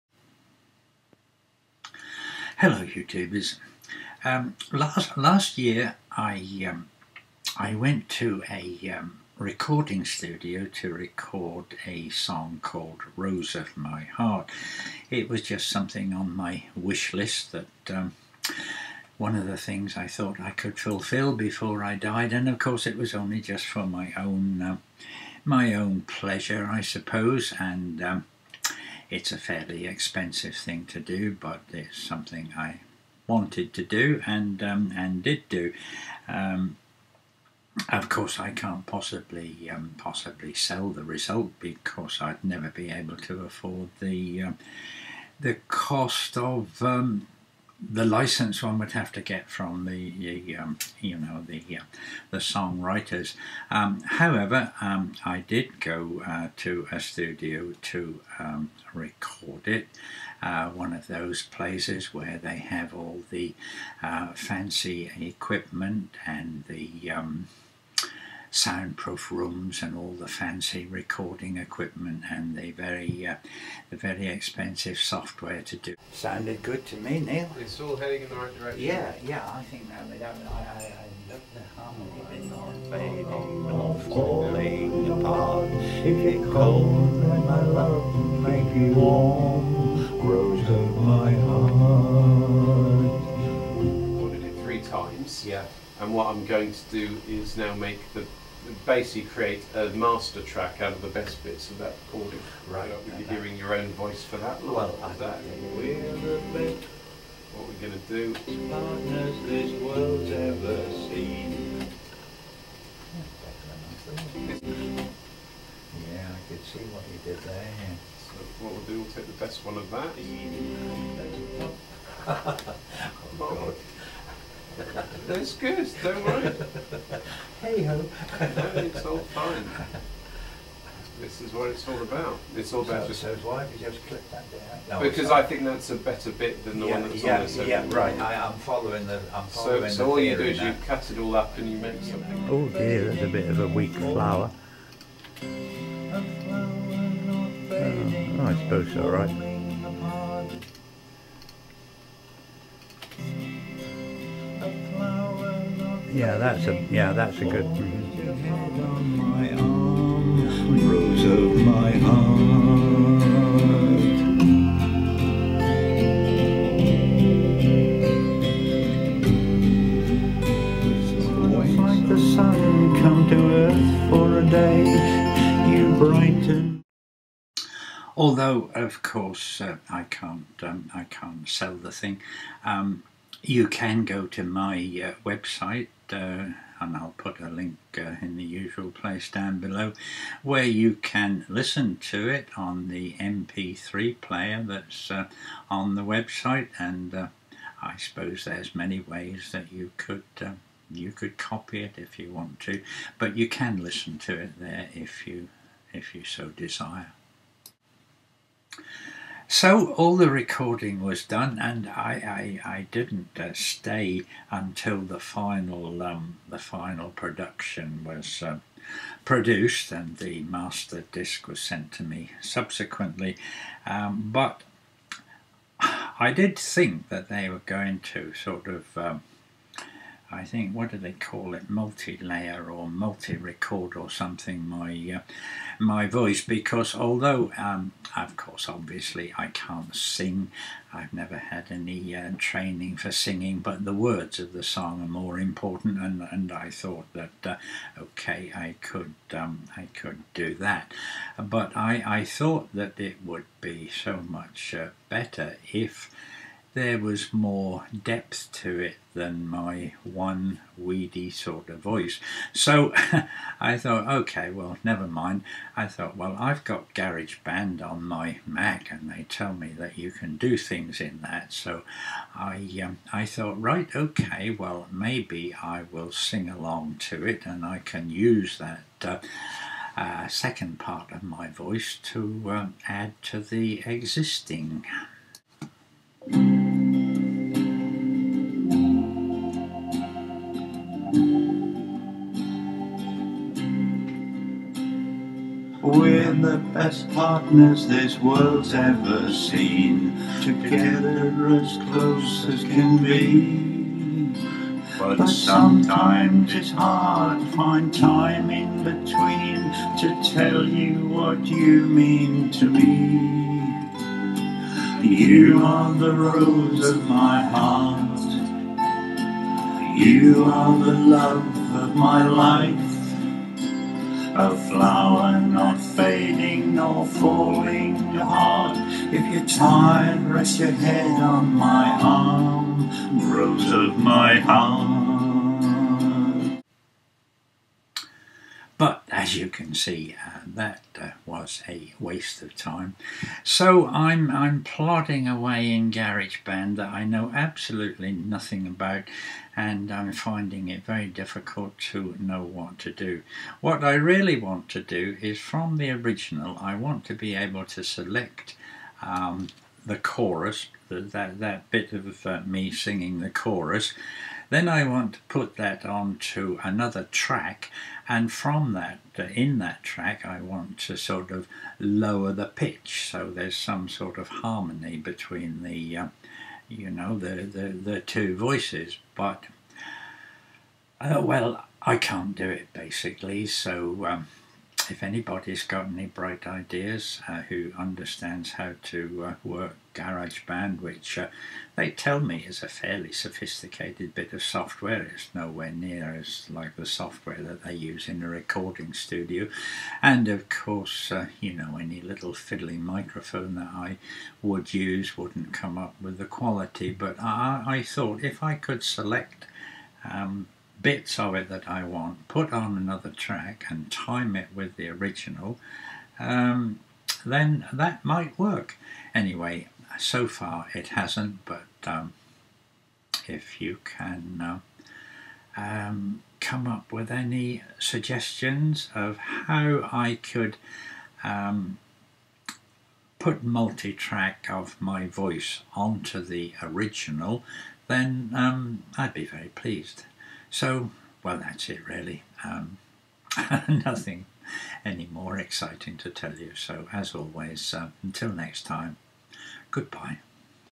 Garage Band Problems
Difficulties of working with the Garage Band software.